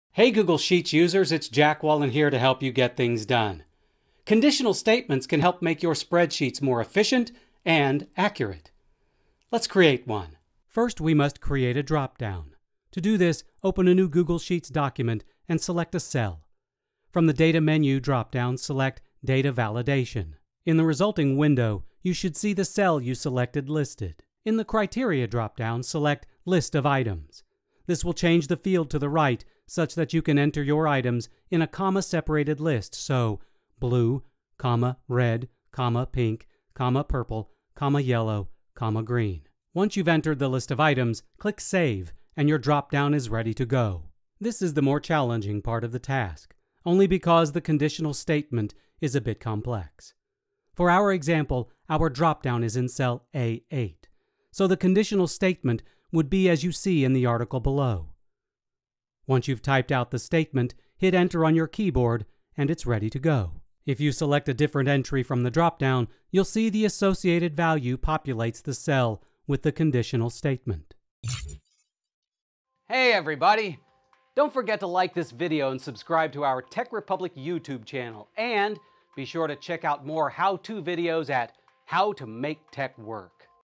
How to create conditional statements for drop-down lists in Google Sheets
Using conditional statements in Google Sheets is an easy way to bring more power and accuracy to your invoices and more.

List of Commands:

=IF(CELL#="NAME",VALUE,IF(CELL#="NAME",VALUE,IF(CELL#="NAME",VALUE)))
=IF(A8="blue",1,IF(A8="red",2,IF(A8="pink",3,IF(A8="pink",4,IF(A8="purple",5,IF(A8="yellow",6,IF(A8="green,7)))))))